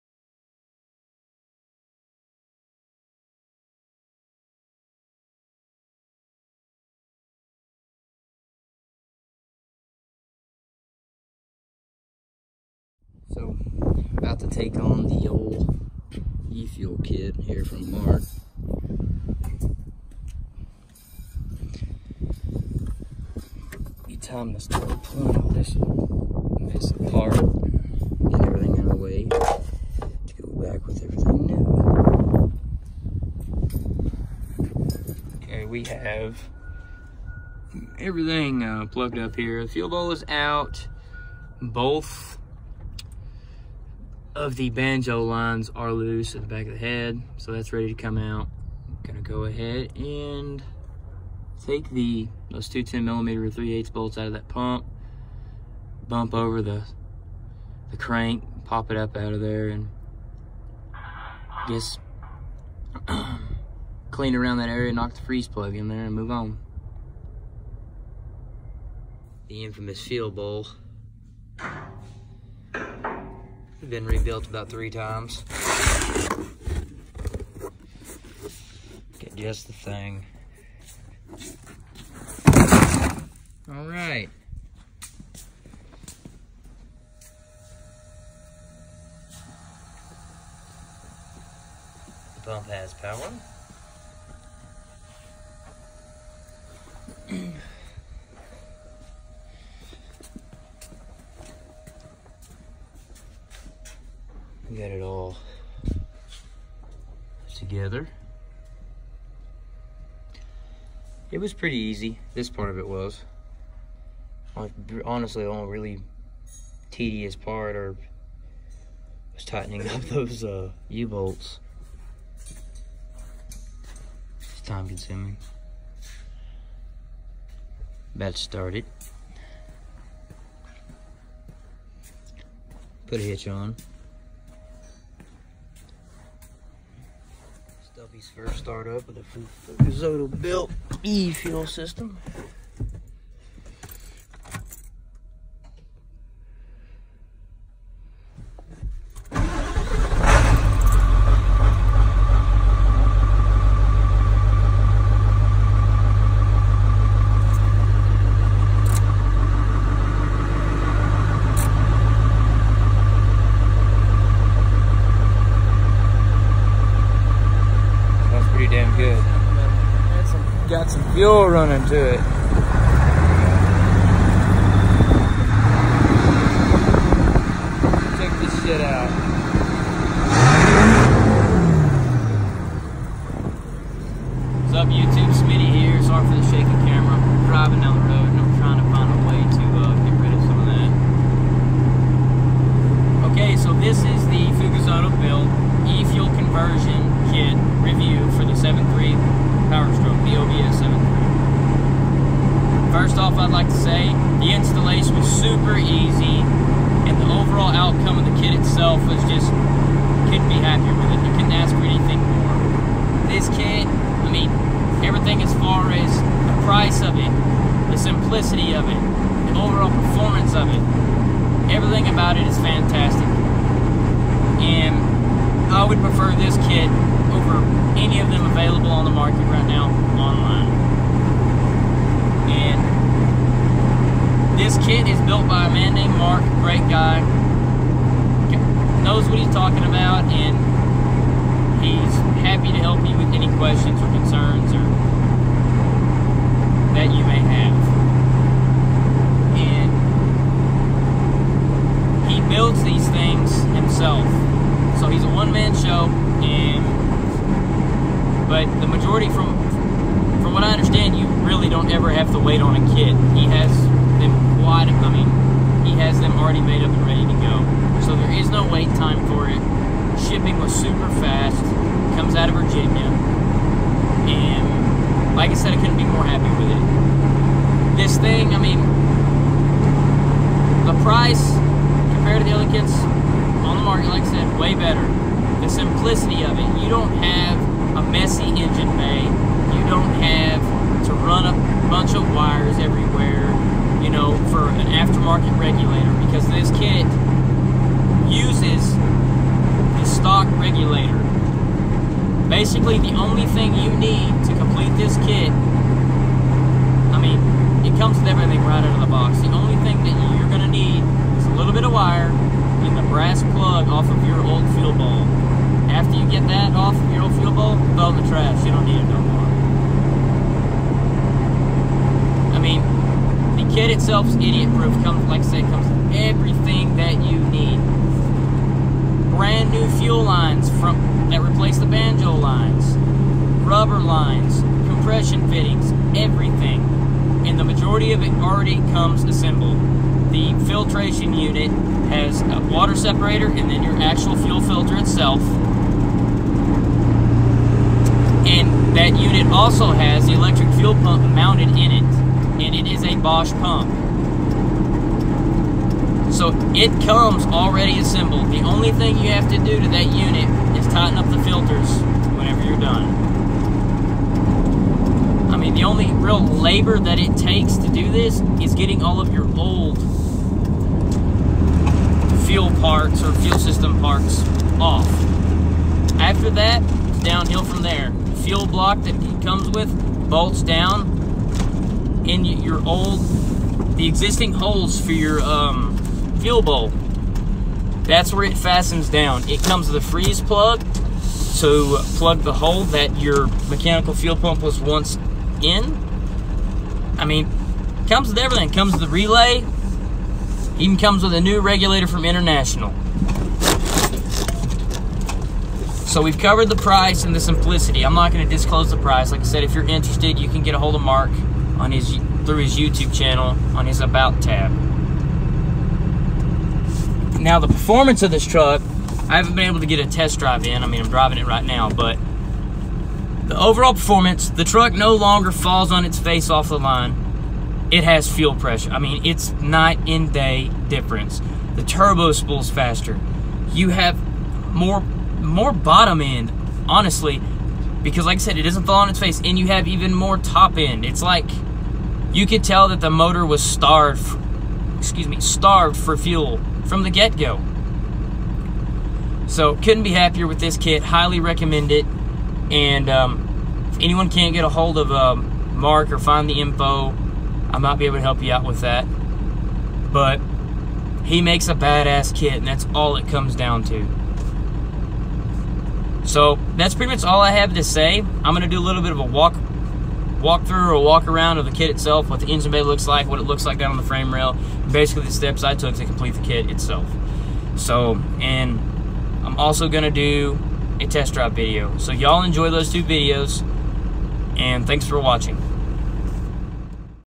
OBS 7.3 PSD Fugazzotto Built E-Fuel Conversion Review
Hopefully this video gives some insight to any reluctant OBS 7.3 owners. The upgrade to an electric fuel system is imperative. Mark’s kit is the way to get it done.